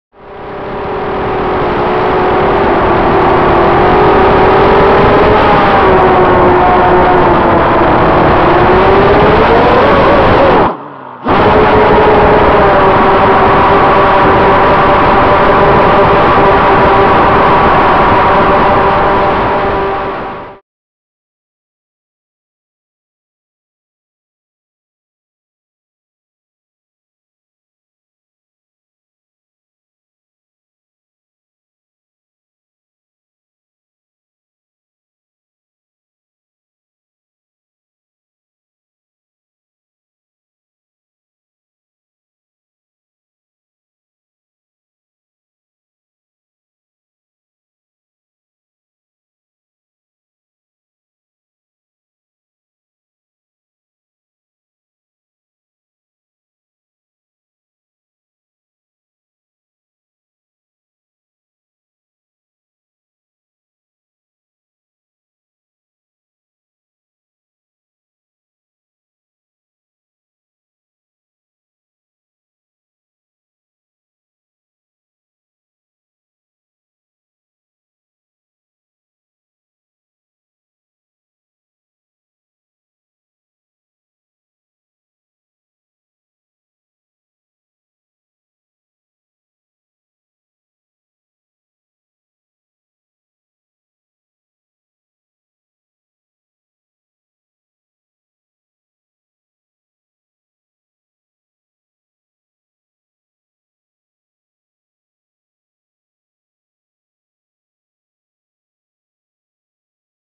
The Rock
The brutal Armattan F1-6 Acro
The Rock near miss
No worries mate,  Armattan isn't affraid of that shit!!